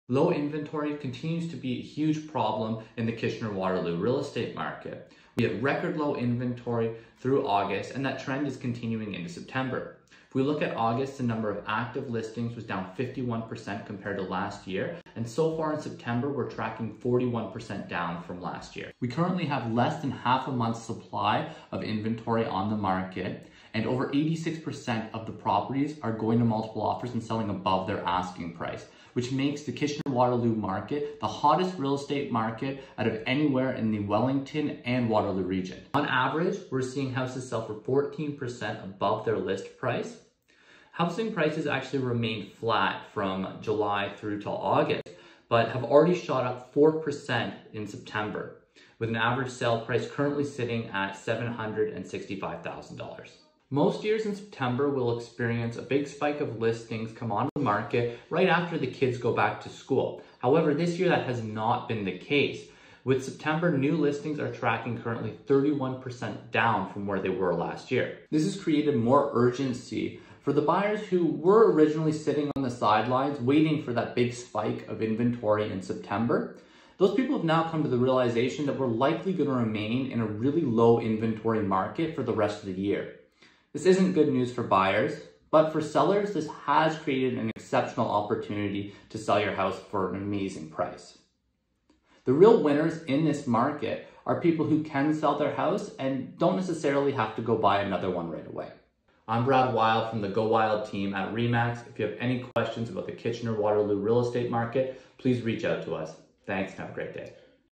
Kitchener/Waterloo Market Update For August 2021
Low inventory continues to be the main issue in the KW market. We've been experiencing record low inventory through August and the trend is continuing in September. The average number of active listings was down 51% in August and so far September is tracking down 41% from last year.

Most years in September, we experience a spike of new listings after the kids go back to school. However, we have not experienced this so far in September with the number of new listings tracking to be down 31% from last year.

This has created more urgency for buyers who were waiting for more inventory as they are coming to the realization that inventory levels will likely remain very low for the remainder of the year. This is bad news for buyers but it has created an exceptional market for sellers.  The big winners right now are people who can sell their home and do not have to buy another one